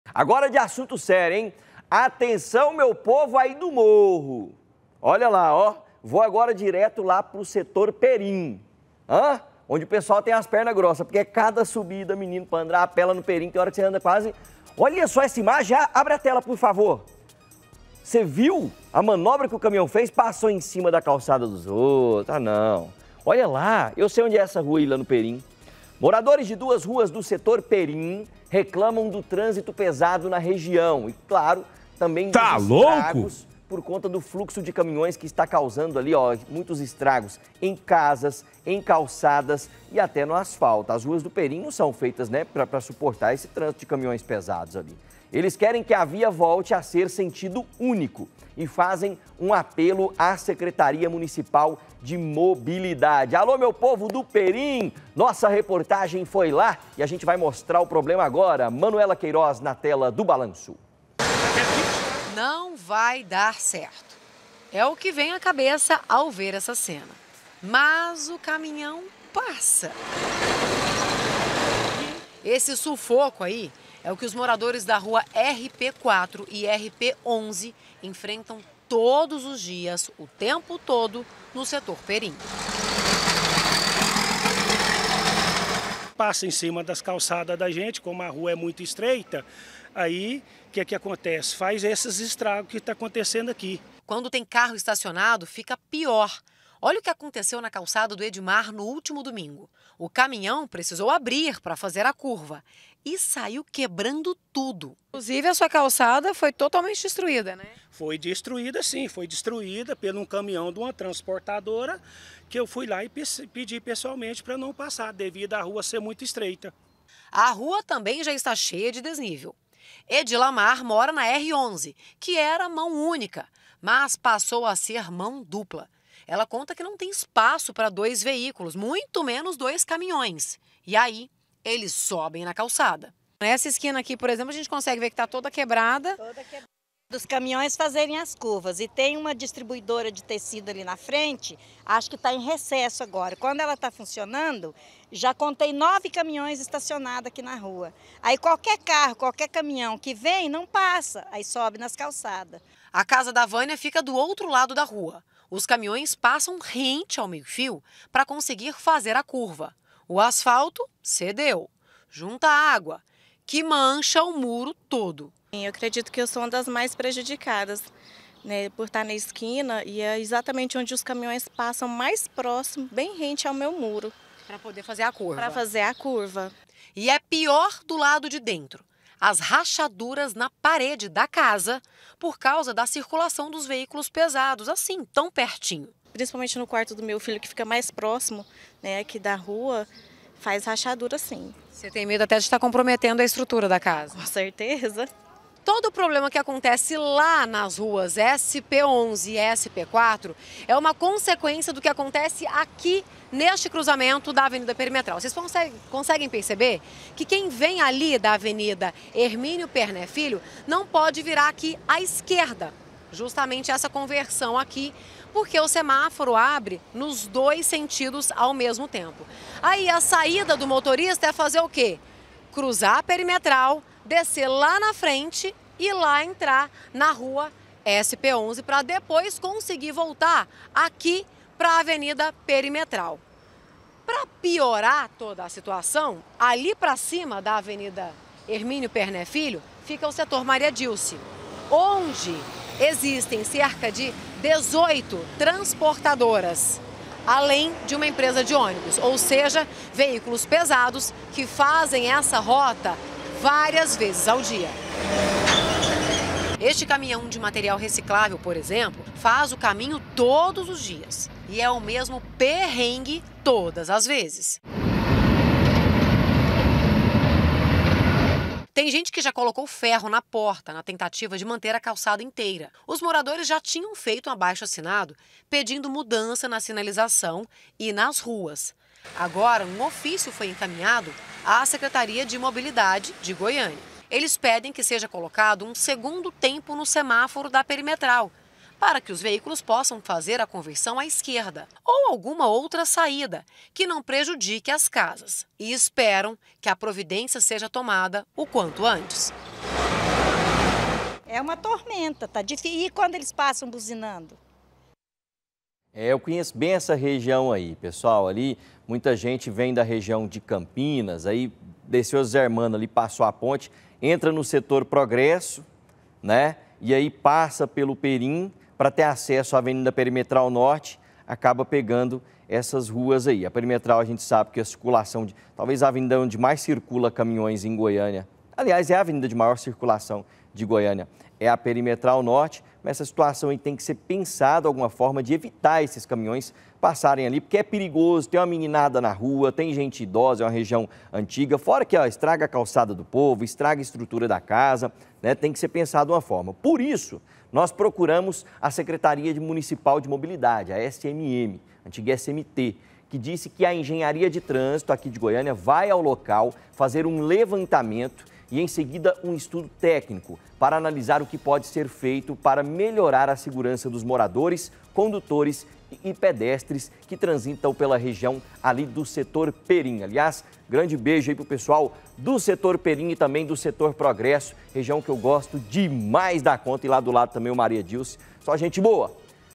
TRÂNSITO E TRANSTORNO: MORADORES RECLAMAM DE ESTRAGOS CAUSADOS POR CAMINHÕES
MORADORES DE DUAS RUAS NO SETOR PERIM RECLAMAM DO TRÂNSITO PESADO NA REGIÃO E DOS ESTRAGOS QUE O FLUXO DE CAMINHÕES ESTÁ CAUSANDO NAS CASAS, CALÇADAS E ATÉ NO ASFALTO. ELES QUEREM QUE A VIA VOLTE A SER SENTIDO ÚNICO E FAZEM UM APELO À SECRETARIA MUNICIPAL DE MOBILIDADE.